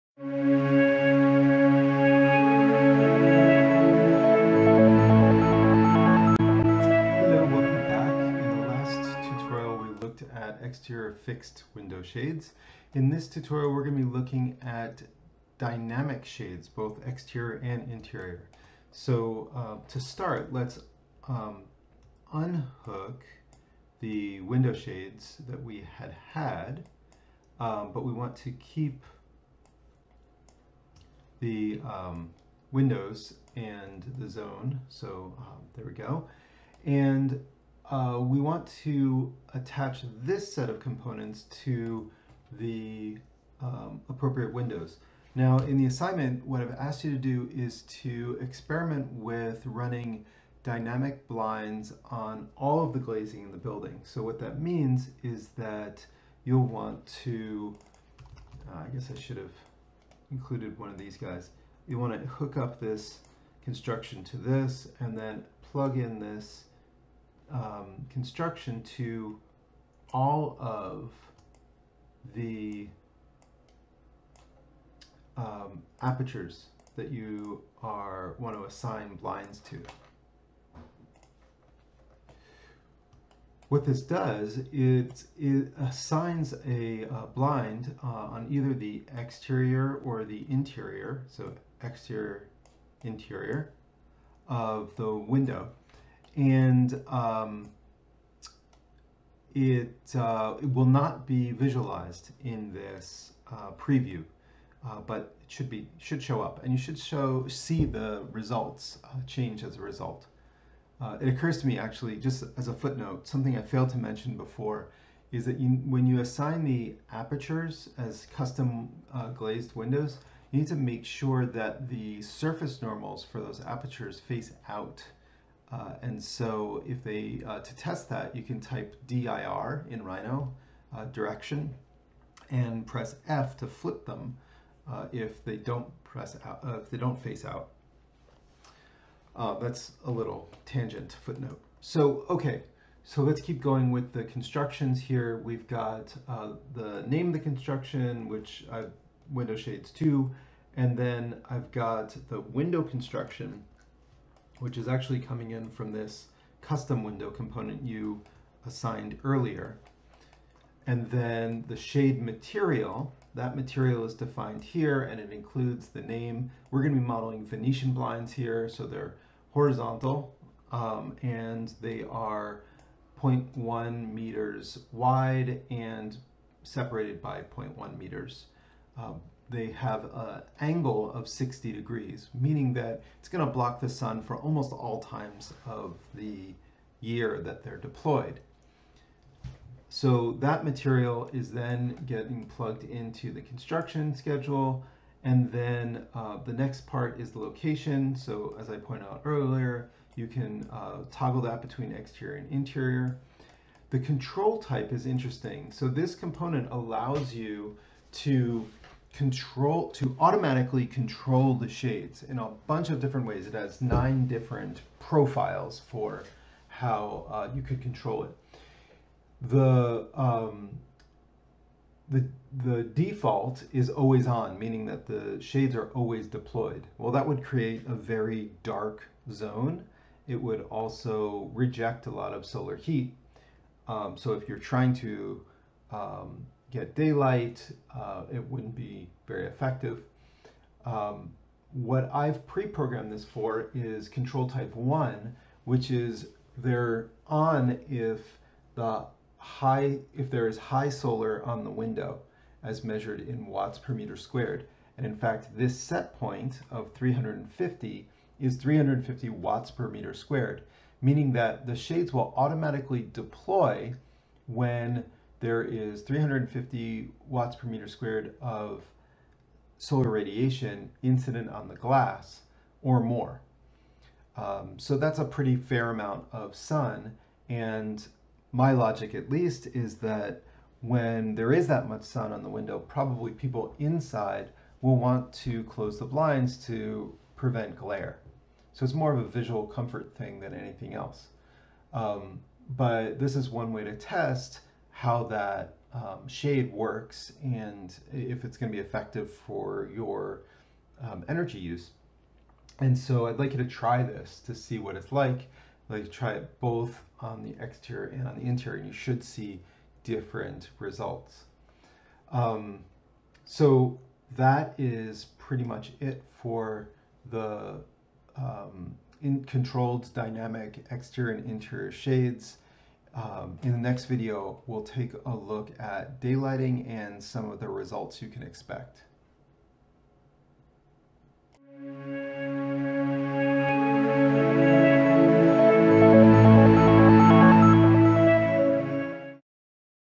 Honeybee V1.4 :: Exterior and Interior Dynamic Blinds [Tutorial 3.4]
This tutorial is intended for use with my class on Regenerative Building Performance at California College of the Arts in San Francisco.